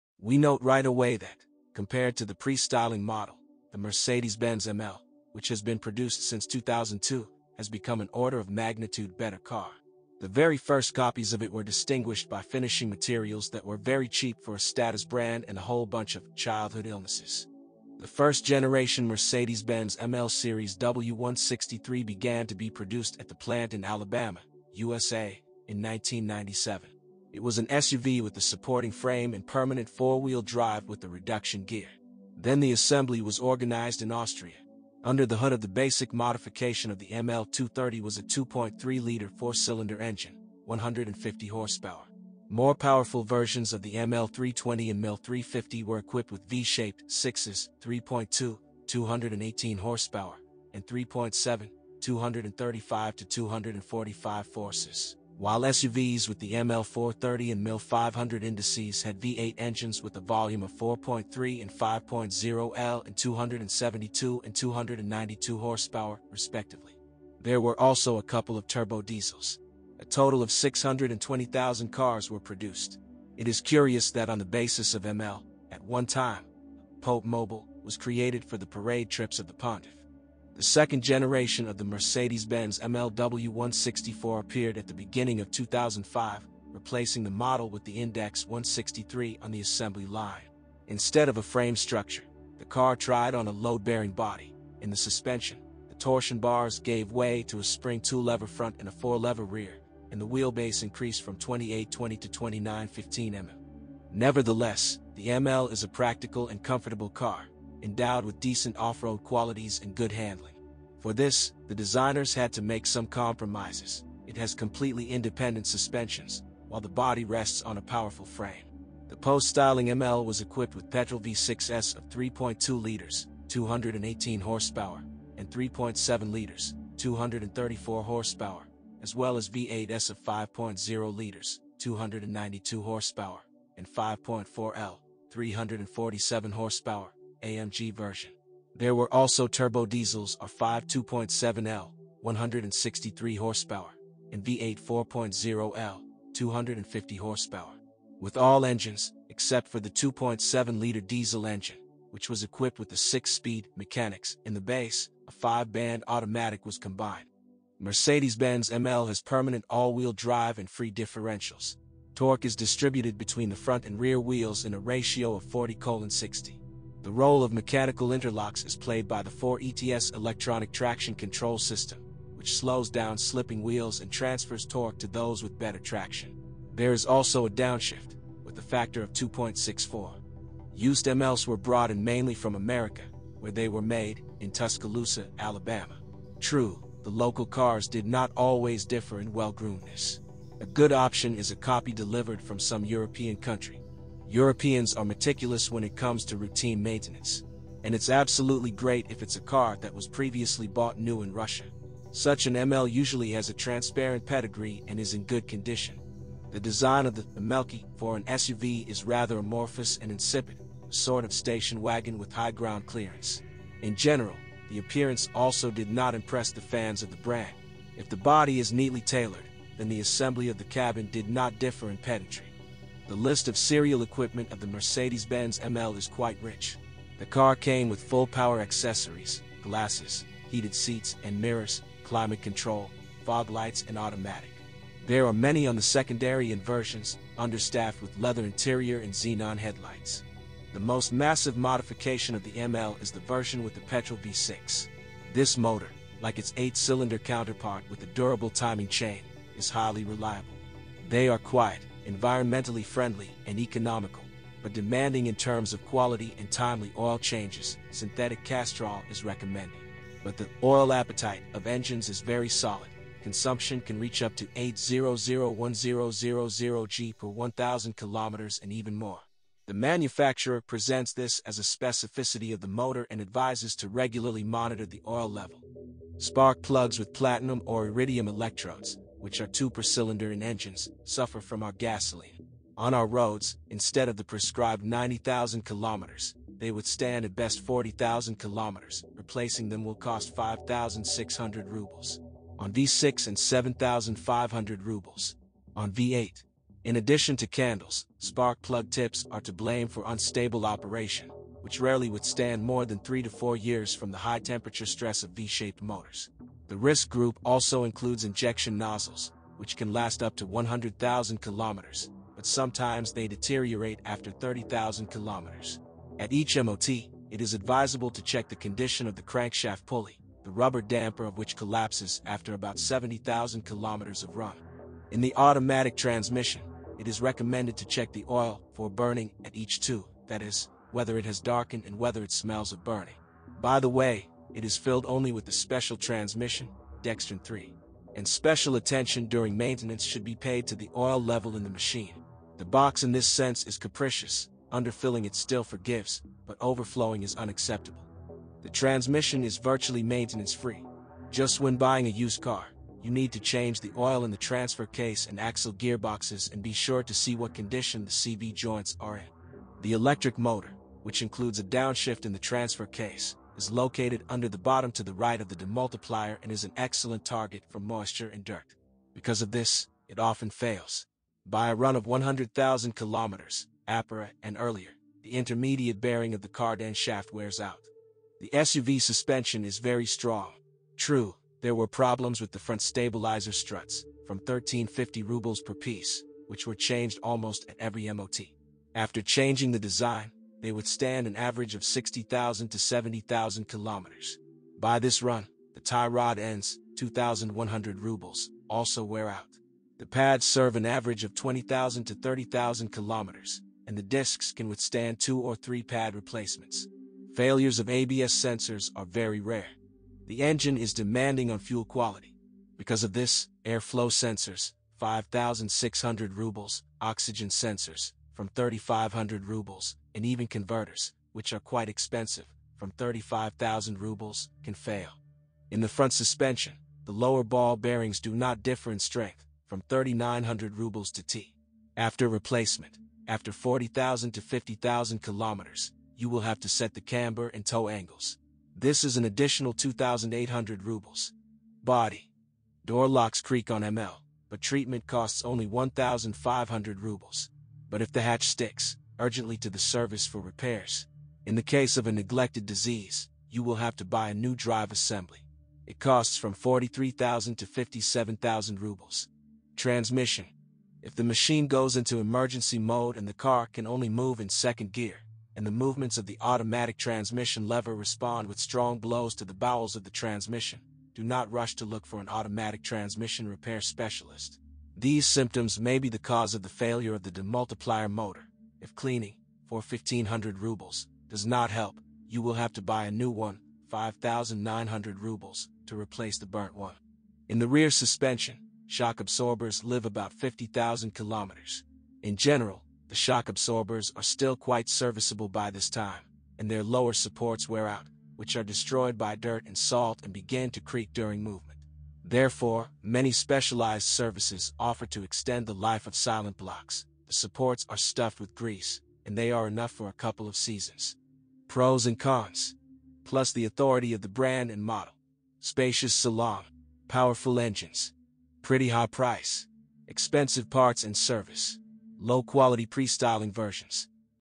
Buying advice with Common Issues Mercedes ML W163
Buying a Used Car Review! Mercedes Benz ML 320 W163 common problems, issues, defects and complaints